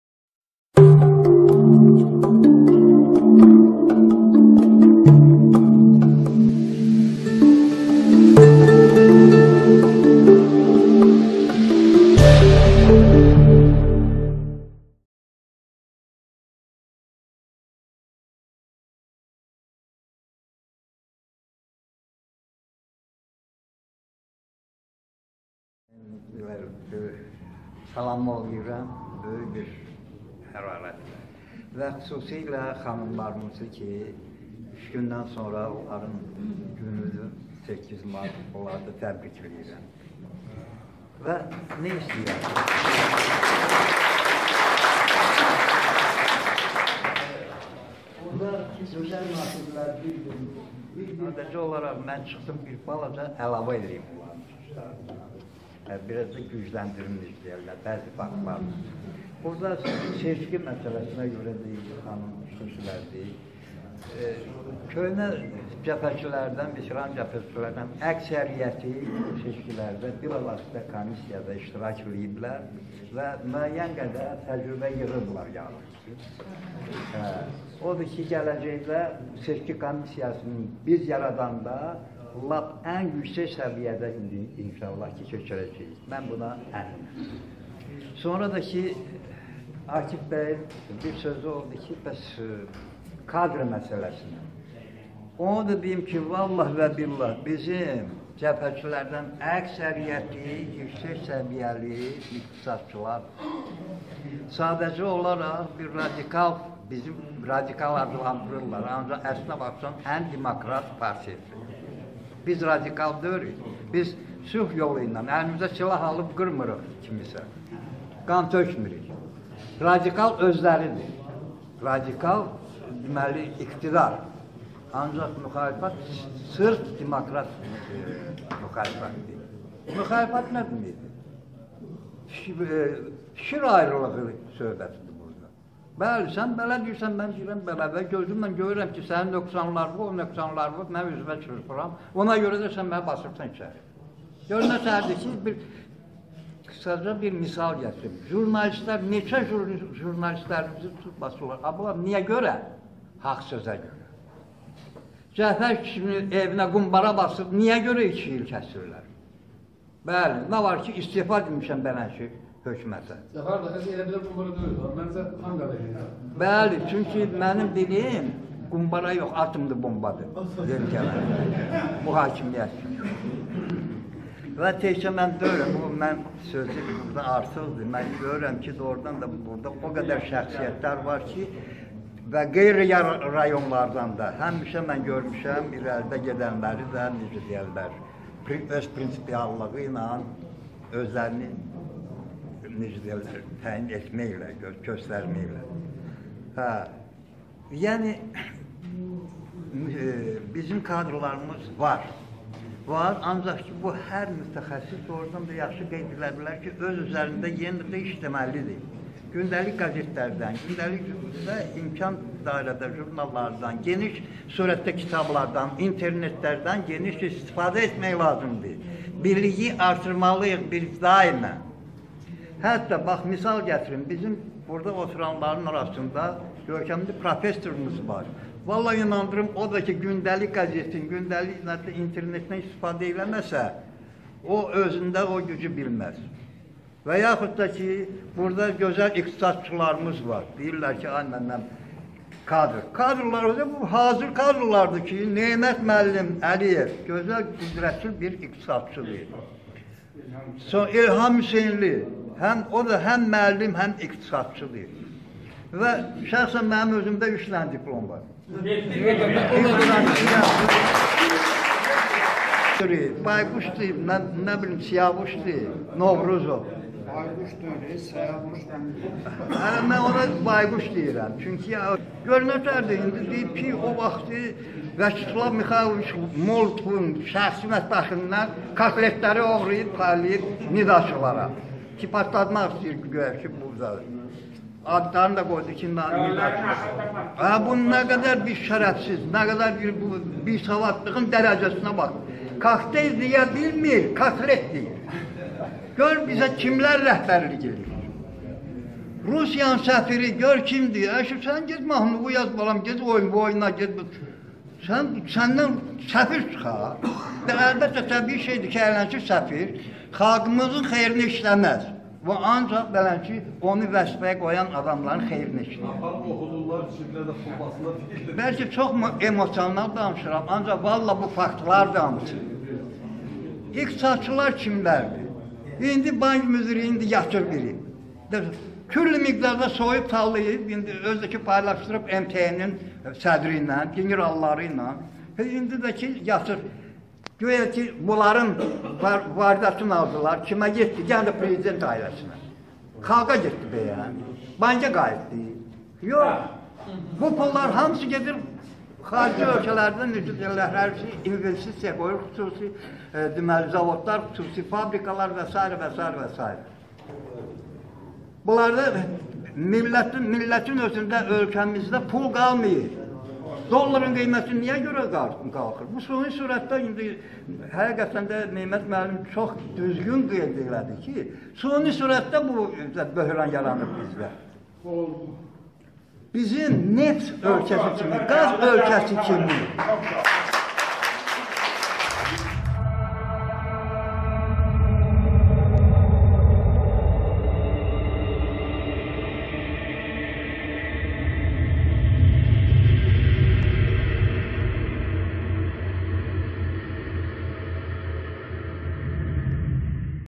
AXCP Binəqədi şöbəsinin müşavirəsi (3-ci hissə)
Veteran cəbhəçimiz Cəfər dayının çıxışı:
Bu gün AXCP Binəqədi rayon şöbəsinin geniş miqyaslı müşavirəsi keçirildi. Tədbirdə ziyalılar və tanınmış insanlar iştirak edirdilər.
video reportajı hazırladı: reportyor Namiq Sadıqlı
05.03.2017